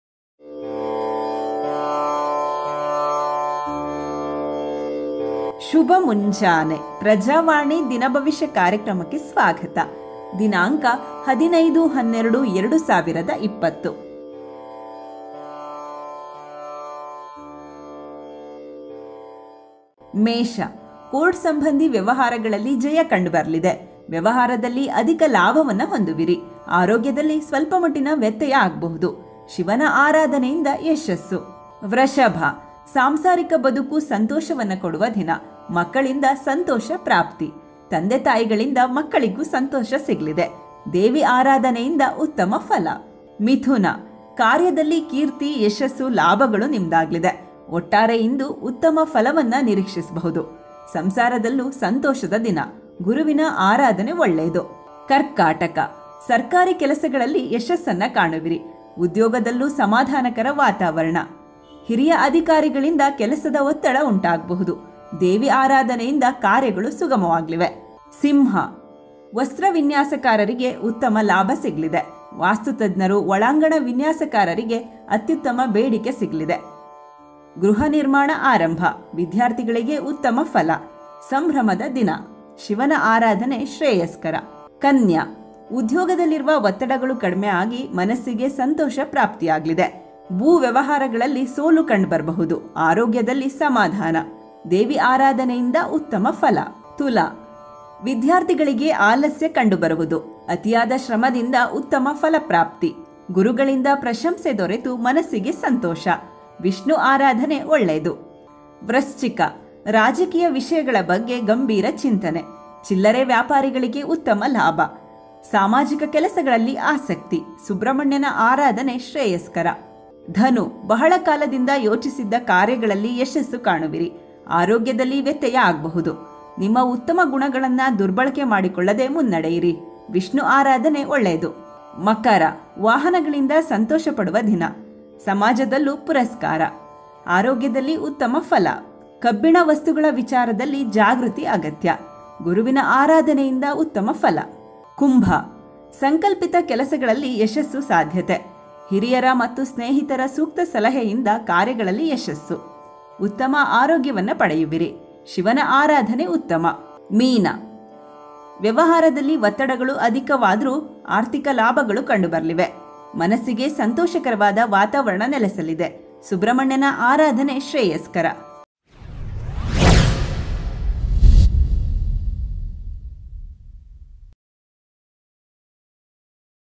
ಈ ದಿನ ಹೇಗಿದೆ?: ನಿಮ್ಮ ದಿನ ಭವಿಷ್ಯ | Todays Horoscope 15 Dec2020
ಈ ದಿನದ ನಿಮ್ಮ ರಾಶಿ ಭವಿಷ್ಯವನ್ನು ಇಲ್ಲಿ ನೋಡಿ. ಯಾವ ರಾಶಿಯವರಿಗೆ ವಿವಾಹ ಯೋಗ? ಯಾವ ರಾಶಿಯವರಿಗೆ ಧನ ಲಾಭ, ಯಾವ ರಾಶಿಯವರಿಗೆ ತೊಂದರೆಗಳು ಎದುರಾಗಲಿವೆ? ನಿಮ್ಮ ರಾಶಿಯ ಇಂದಿನ ಭವಿಷ್ಯ ಹೇಗಿದೆ ತಿಳಿಯಿರಿ. Today Horoscope 15 Dec, 2020

ಮತ್ತಷ್ಟು ವಿಡಿಯೊಗಳಿಗಾಗಿ: Youtube.com/Prajavani
ತಾಜಾ ಸುದ್ದಿಗಳಿಗಾಗಿ: Prajavani.net ನೋಡಿ
ಫೇಸ್‌ಬುಕ್‌ನಲ್ಲಿ ಲೈಕ್ ಮಾಡಿ: Facebook.com/Prajavani.net
ಟ್ವಿಟರ್‌ನಲ್ಲಿ ಫಾಲೋ ಮಾಡಿ: Twitter.com/Prajavani